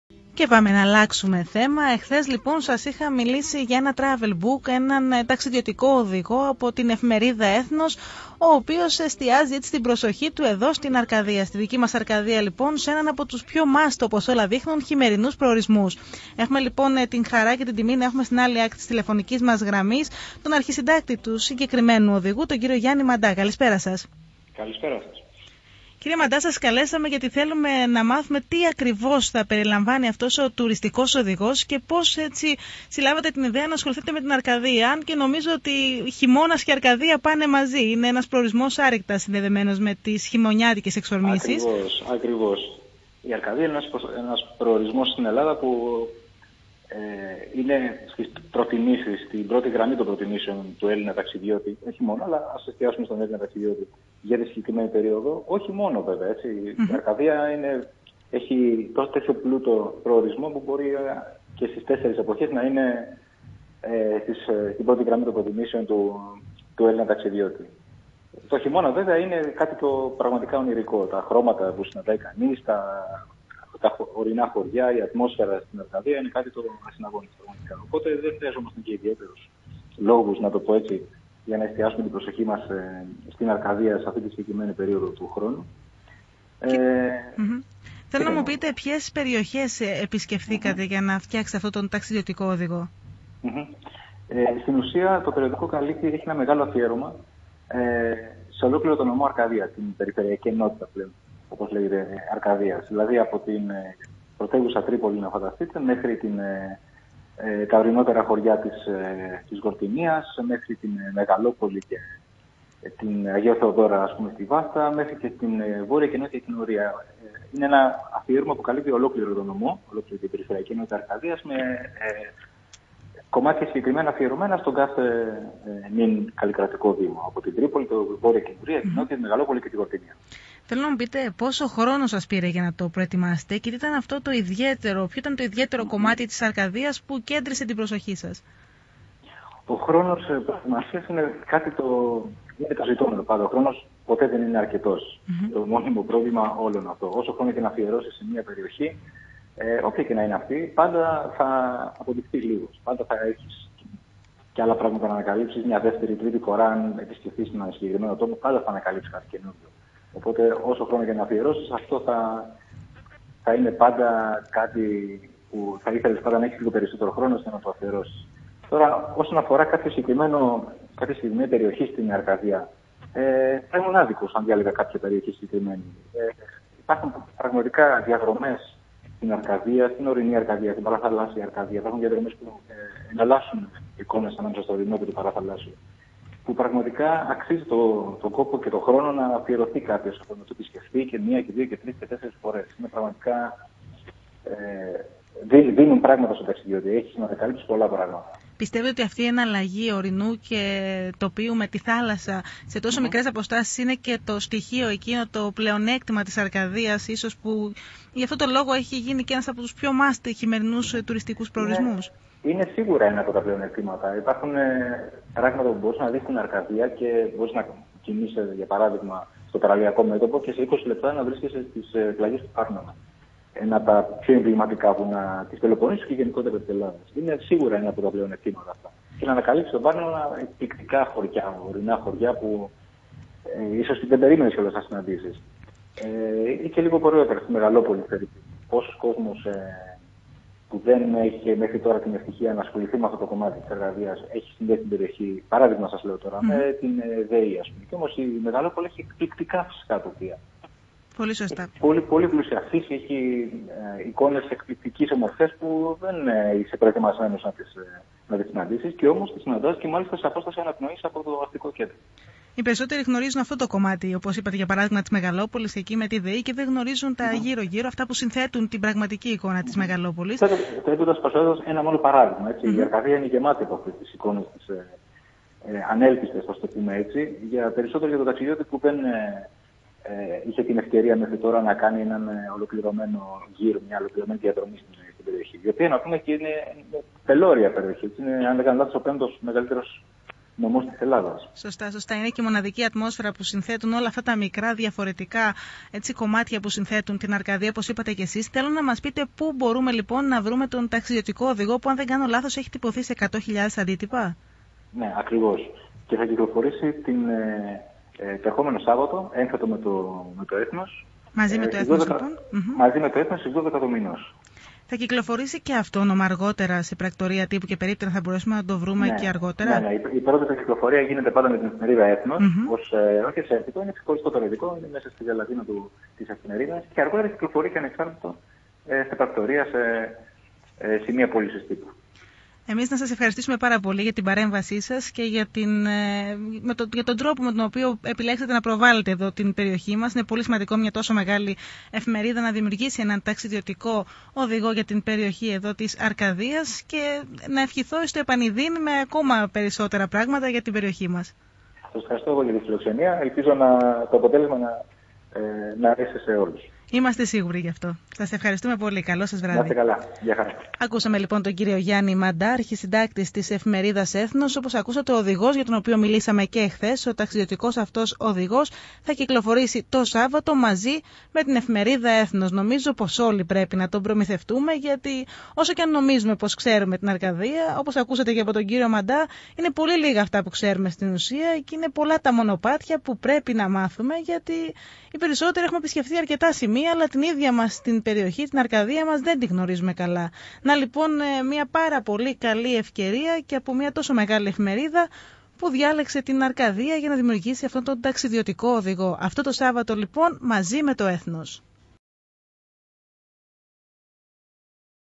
Γιατί το "ΕΘΝΟΣ" επέλεξε την Αρκαδία
Ο Αρχισυντάκτης του travelbook στον arcadia 93.8